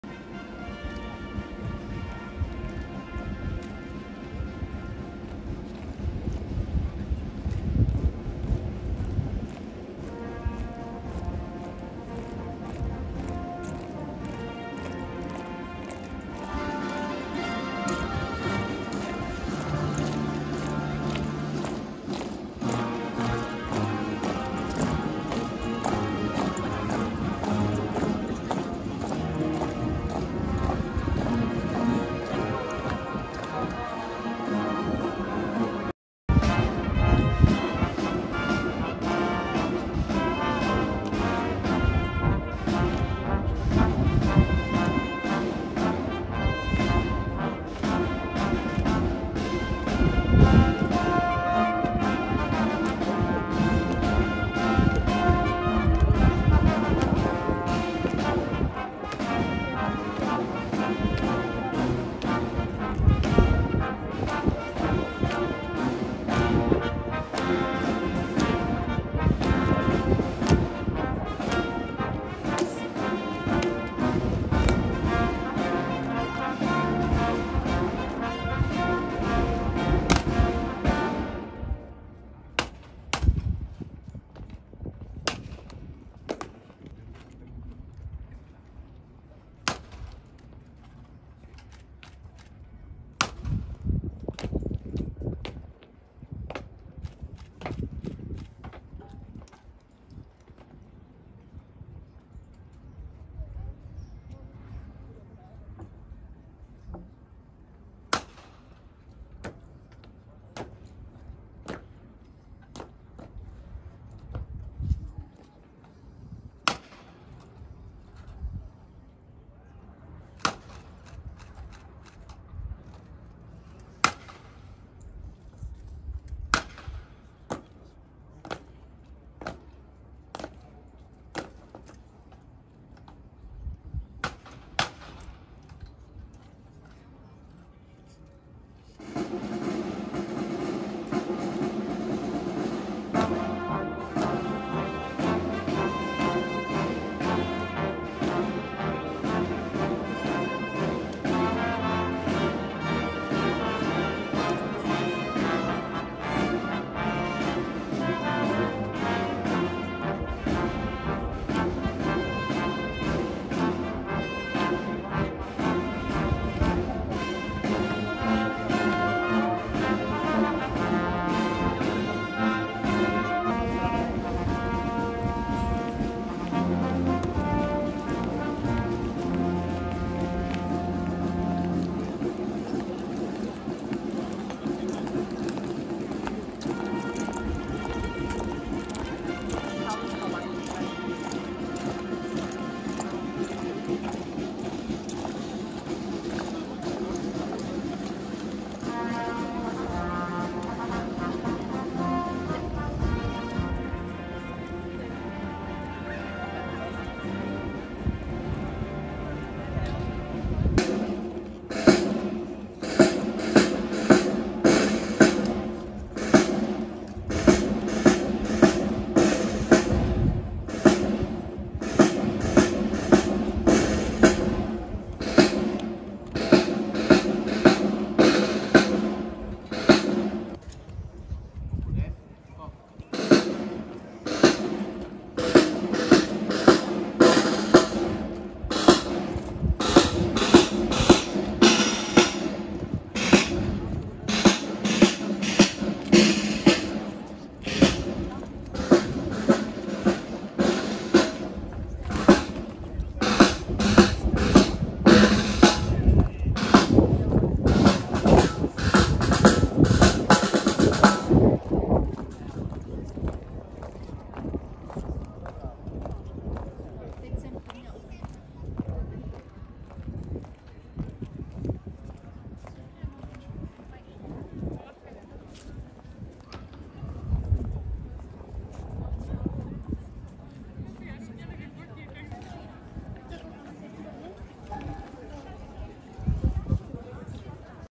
Changing of the guards in Prague Castle
Changing of the guards in Prague castle is a must see